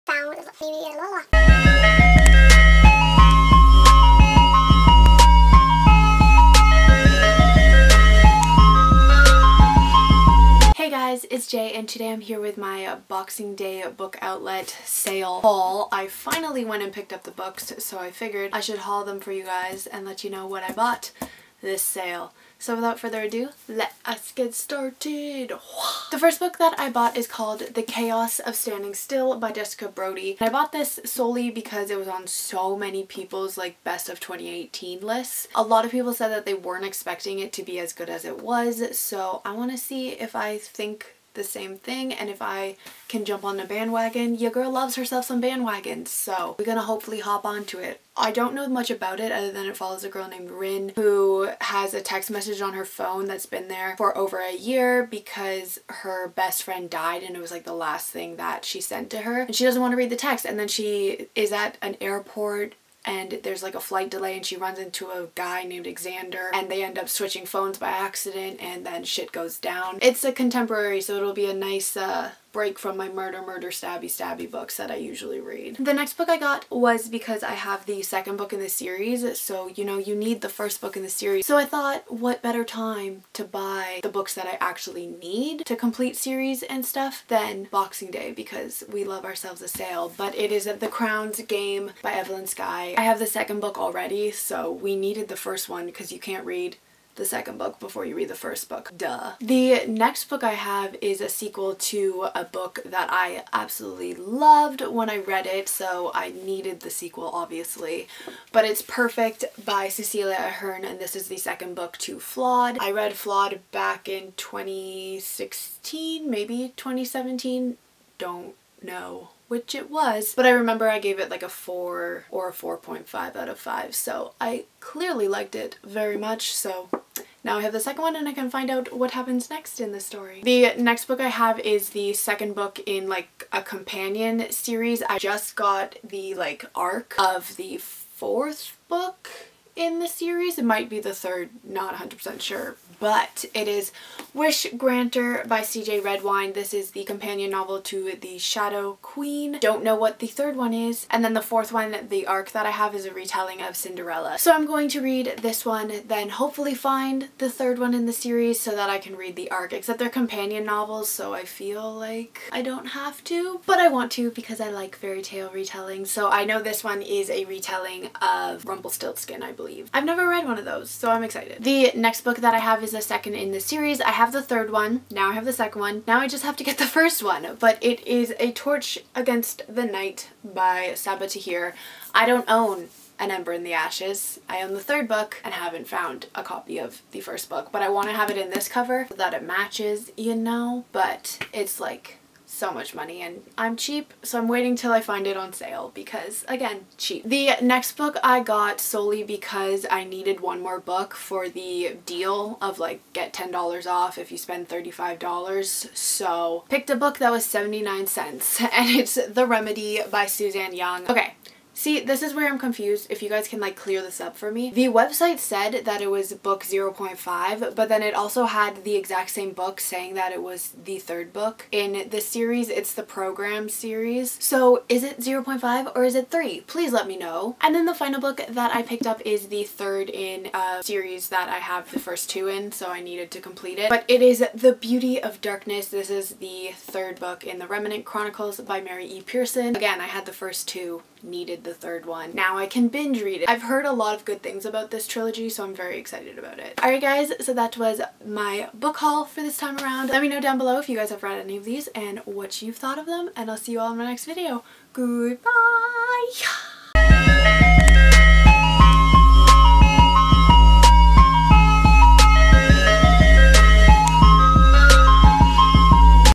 BOXING DAY SALE?! | FETUS JAY FRIDAY
This was filmed back in 2019... I am aware of Book Outlet and their views, and I do not agree with them in any way.

I've read The Chaos of Standing Still (4/5 Stars), Perfect (3/5 Stars), The Wish Granter (4.5/5 Stars), and the entire Ember in the Ashes Series (4.5/5 Stats; 4.5/5 Stars; 5/5 Stars; 5/5 Stars)